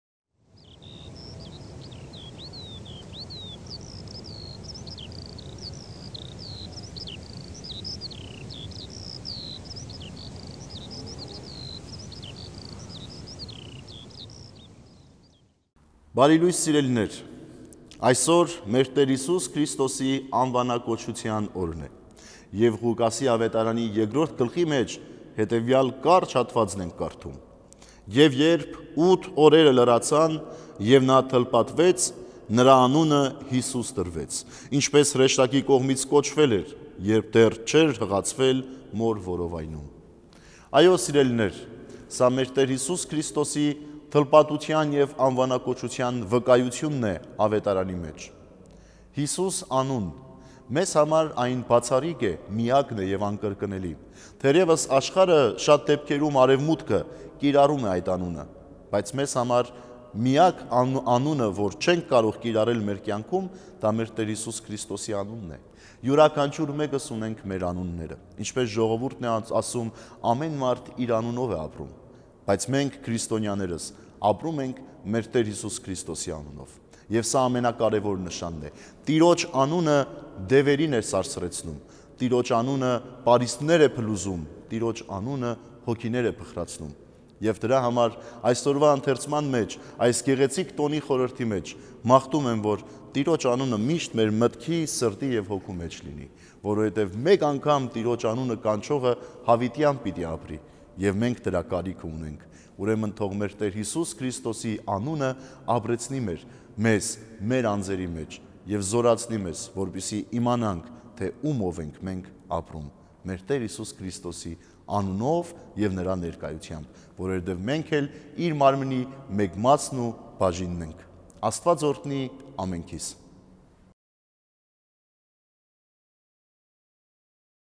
Հունվարի 13
2022թ. հունվարի 13-ի օրվա խորհուրդը (13 † Եշ. ԳՁ. Ը օր Ս. Ծննդեան: Տօն Անուանակոչութեան Տեառն մերոյ Յիսուսի Քրիստոսի: Կող. 2.8-15: Ղուկ. 2.21:) մեկնում է Տ. Առնակ քահանա Հարությունյանը:

January 13

Sacrament of January 13, 2022 (13 Еighth day of Nativity. Feast of Naming of Our Lord Jesus Christ. - Col. 2.8-15 Luke 2.21). Rev. Fr. Arnak Harutyunyan speaks on the sacrament of the day.